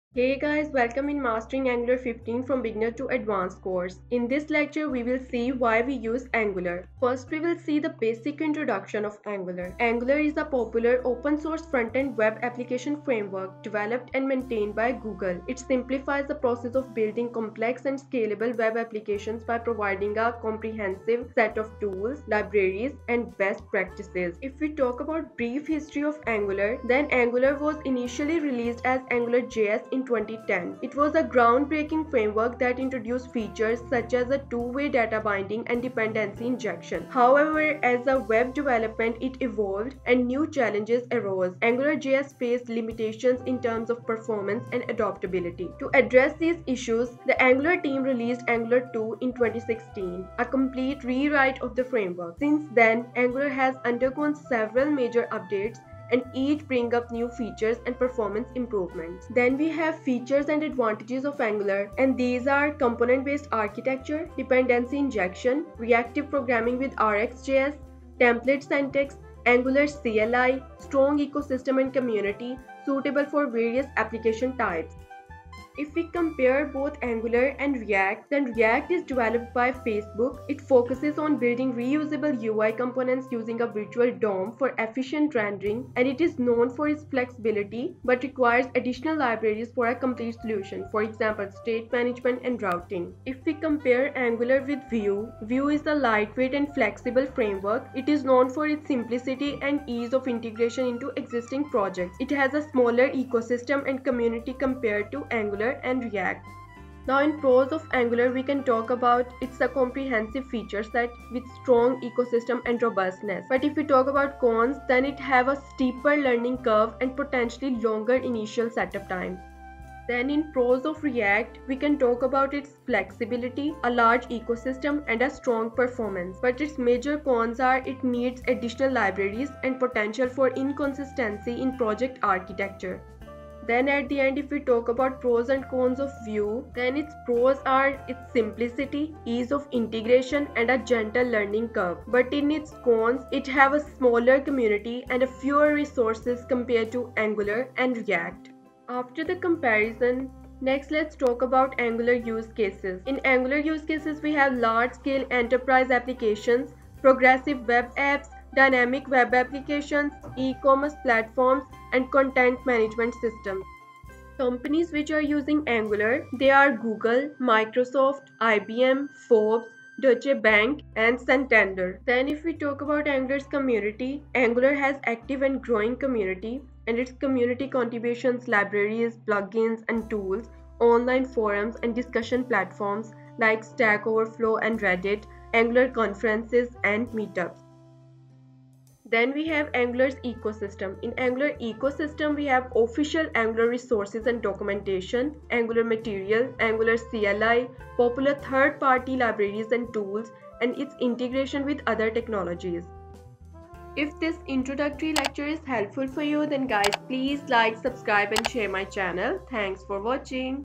Why use Angular? | Mastering in Angular 15 Beginner to Advance Course | Lecture 1
Angular is a popular front-end web development framework created and maintained by Google. It is an open-source framework that uses TypeScript, a superset of JavaScript, and follows the Model-View-Controller (MVC) architecture pattern.
Some of the features and capabilities of Angular include two-way data binding, dependency injection, components, directives, pipes, routing, and testing.
Angular provides a seamless way to synchronize data between the model and the view. Any changes made to the model are automatically reflected in the view and vice versa.
Angular uses a dependency injection system that helps to manage and organize code more effectively.
Angular applications are built using reusable and modular components that can be easily combined to create complex user interfaces.
Directives are special attributes that can be added to HTML tags to provide additional functionality, such as hiding or showing elements based on certain conditions.
Pipes are used to transform data in templates, such as formatting dates or converting text to uppercase.
Angular provides a powerful routing system that allows for easy navigation between different views in an application.
Angular has built-in support for testing, making it easy to write and run unit tests for your application.
Overall, Angular is a powerful framework that enables developers to build complex, scalable, and performant web applications with ease.